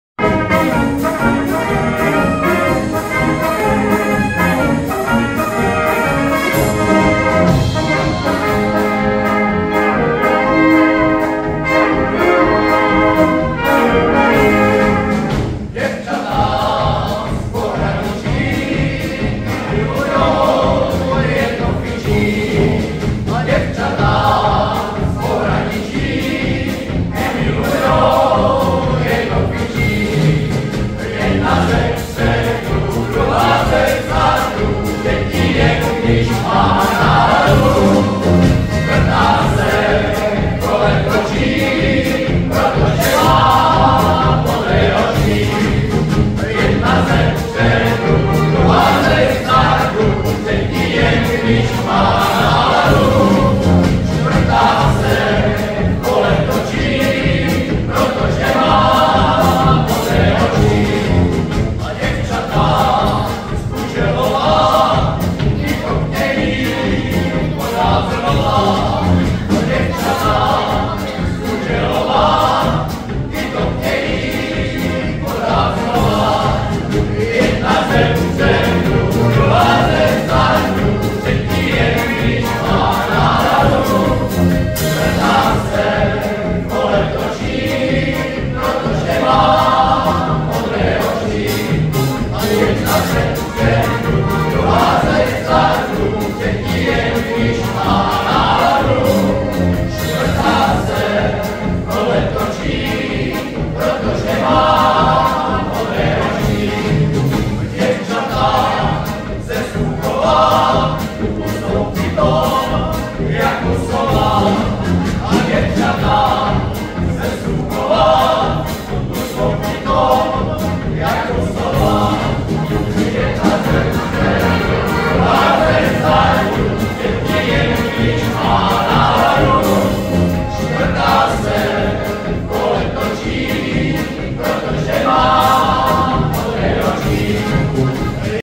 LOUKA-Ukázka závěrečného veselého výstupu DH LIPOVJANKY při 12. setkání lidových vypravěčů a bavičů
- 1. dubna 2023, 20.20 hodin, KD  v Louce (Horňácko)
- natáčeno na mobil Samsung Galaxy ULTRA 22